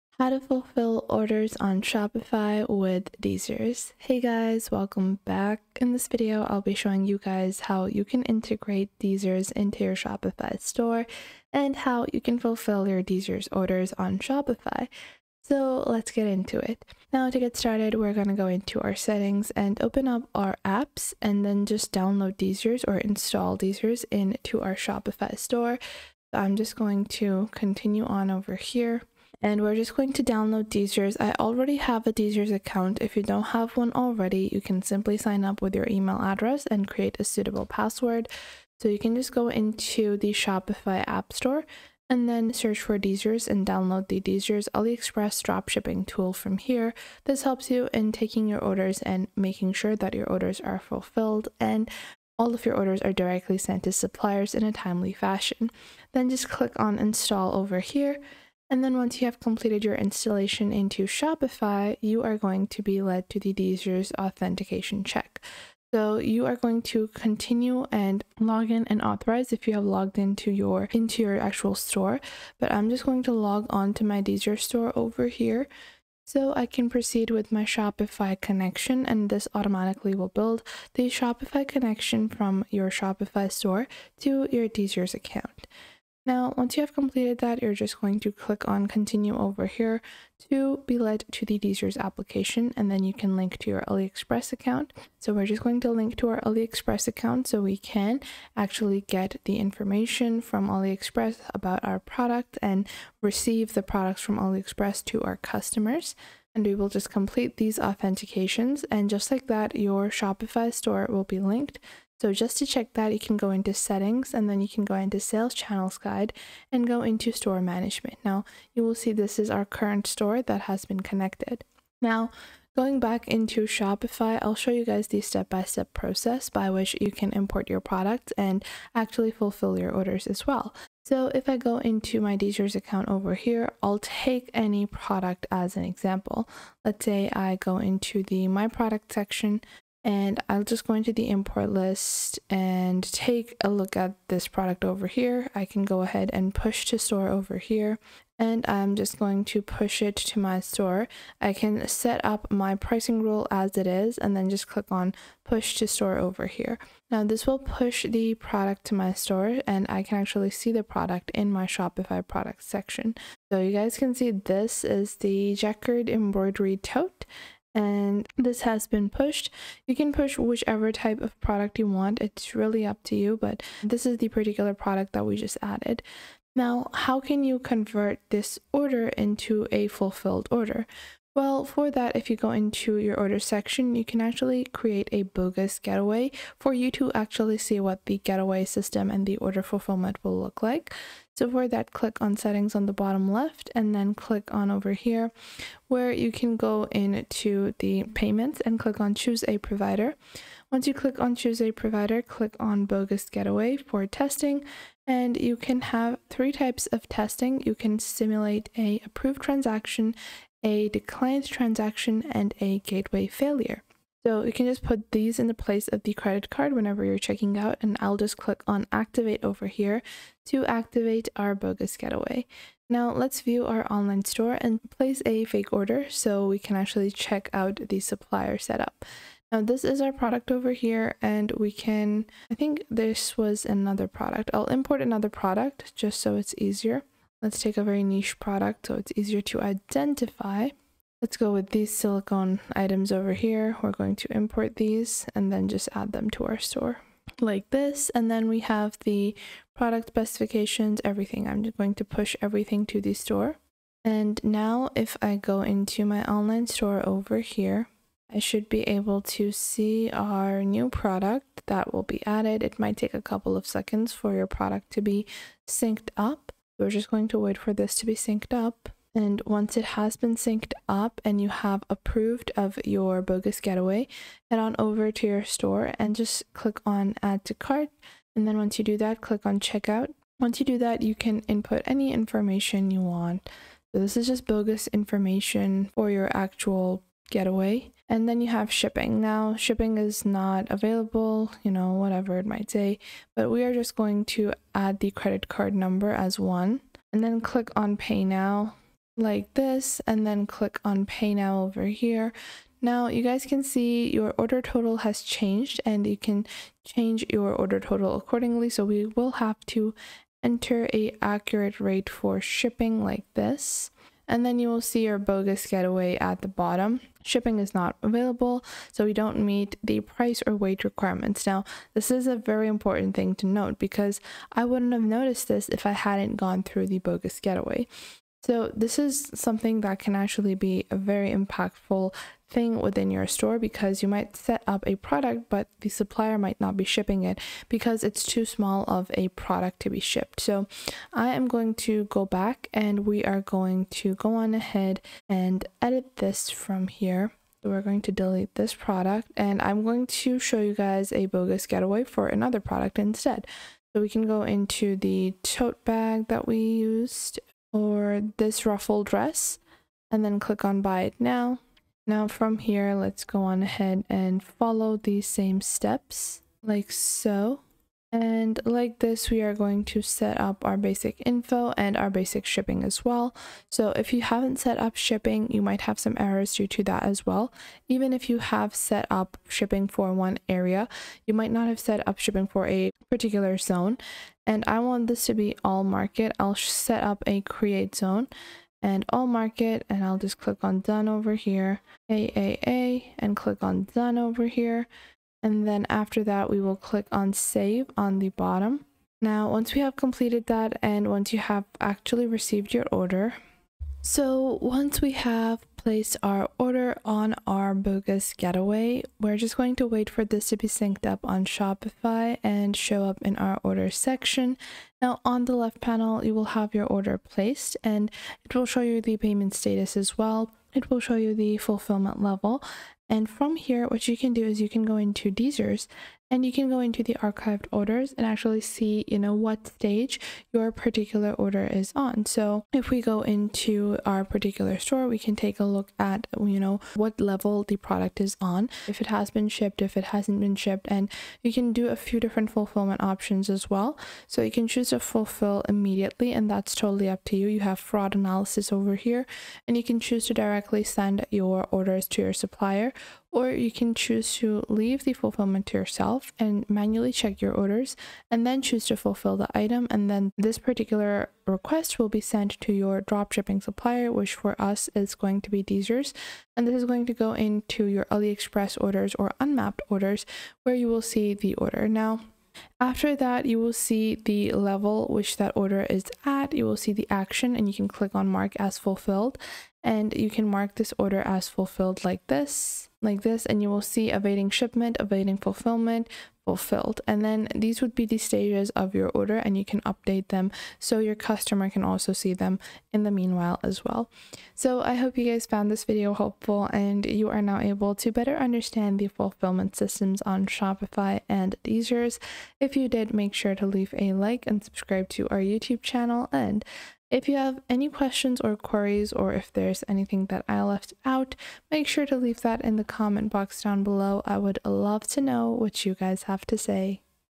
How To Fulfill Orders On Shopify DSers 2025 (For Beginners)
In this video we show you How To Fulfill Orders On Shopify DSers. This will cover everything that you need to know in order to use a Chatbot in your Shopify Store.